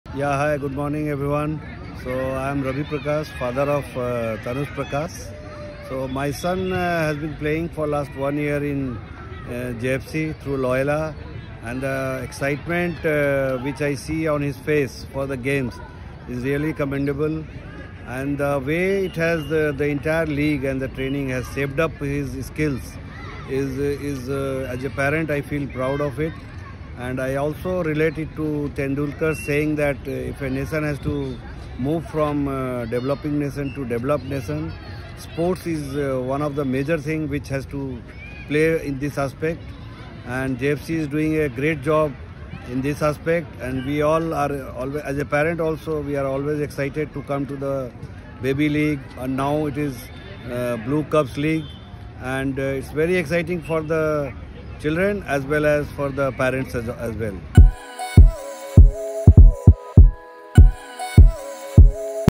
Ravi Prakash shares how Jamshedpur FC Blue Cubs is shaping the future of football! ⚽️👨‍👦
Ravi Prakash, father of Tanush Prakash from Loyola School, shares how Jamshedpur FC Blue Cubs is shaping the future of football! ⚽️👨‍👦